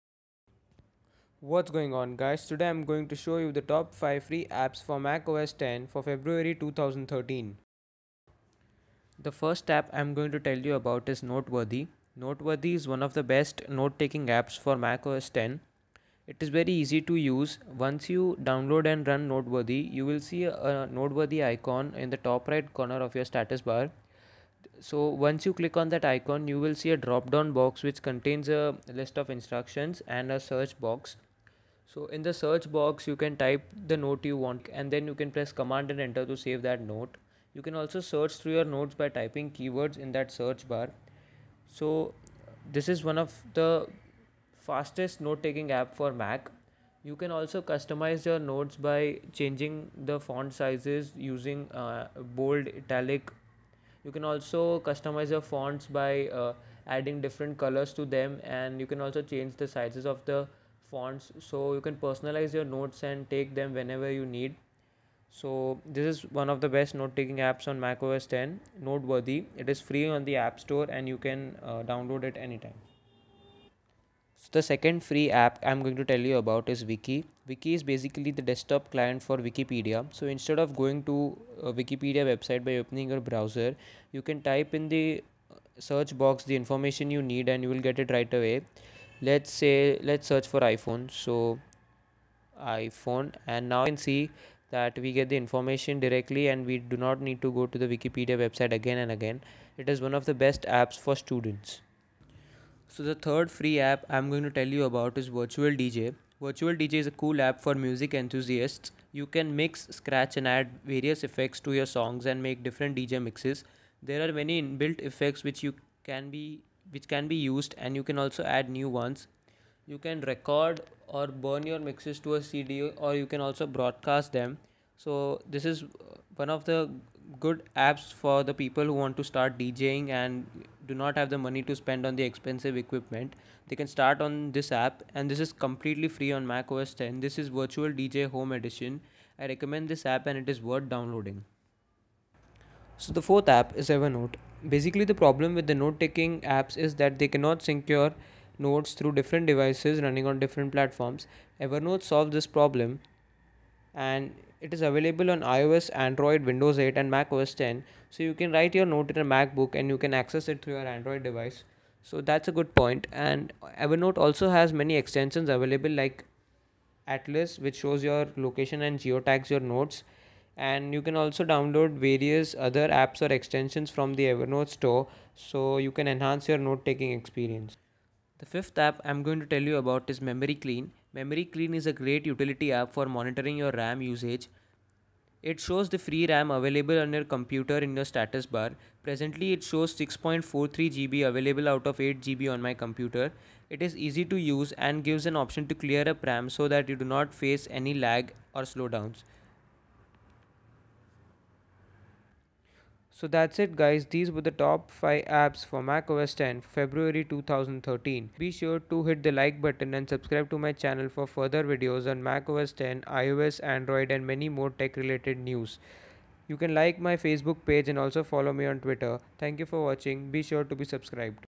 Top 5 Free Mac OS X Apps February 2013
Sorry, for not uploading in 720p.All the videos will be HD in future.
In this Video,I mention the top 5 free apps for Mac OS X for February 2013
1.Noteworthy
2.Wiki
3.VirtualDJ
4.Evernote
5.Memory clean